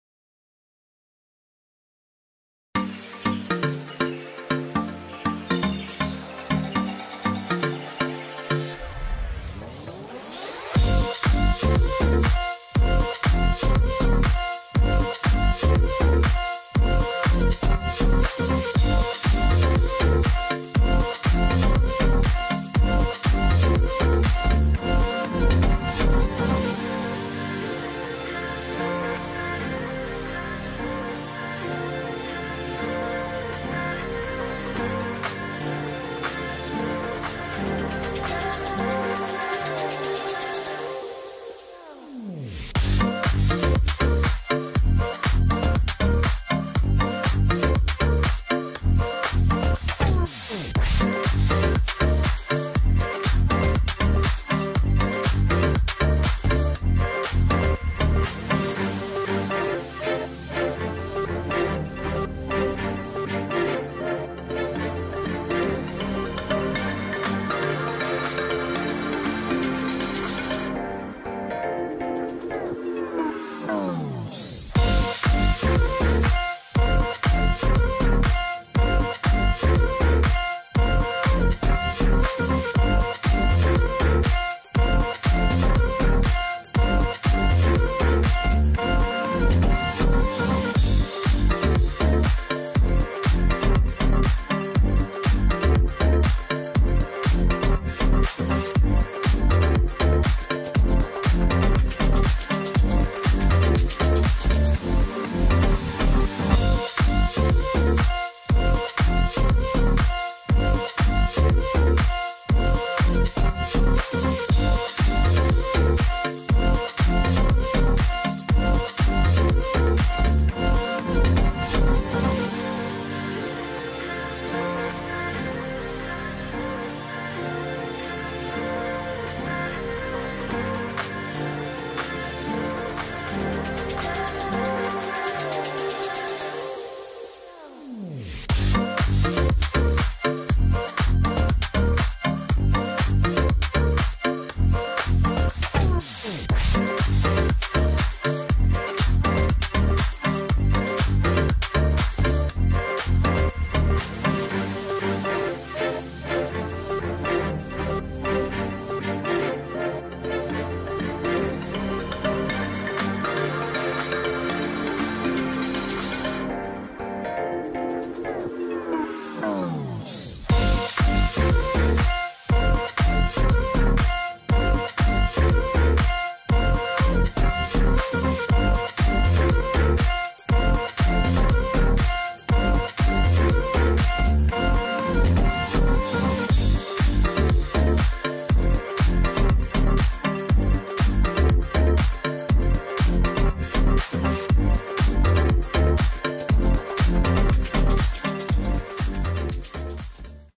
Dhanoas kid tricycle shelly assembly steps
Brand -Dhanoas
Model- shelly
This trikes has the best seating position for kid and uses the beat quality heavy frame with virgin quality plastic material for accessories and tyres. It comes with parental control so the parent can easily steer the trike according to their comfort.